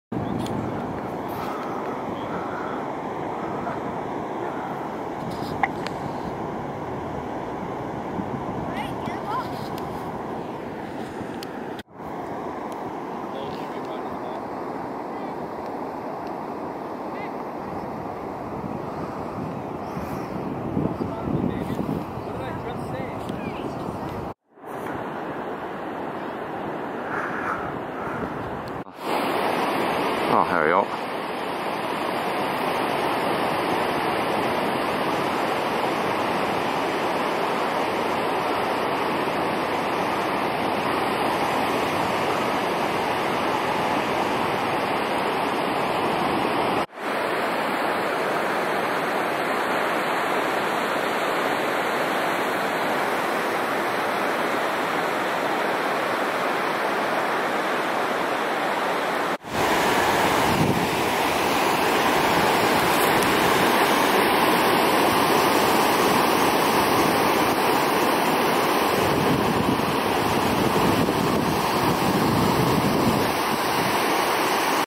Fog At Long Beach, B.C.
So we went out to Long Beach just outside of Tofino on Vancouver Island.
The fog that day made a magical impression to the right kind of mind.